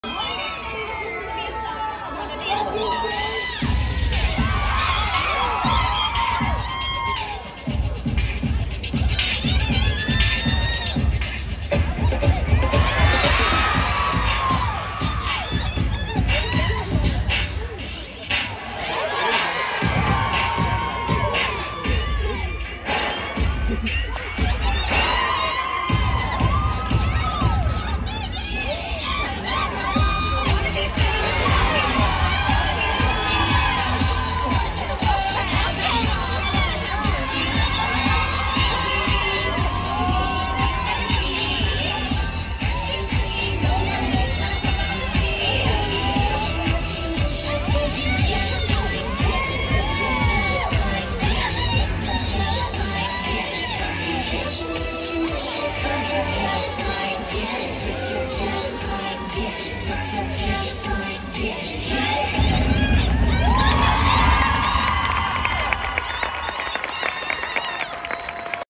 Northwestern State University Homecoming LipSync PepRally Spring 2021
Demon Dazzlers performance during the annual Homecoming Lipsync battle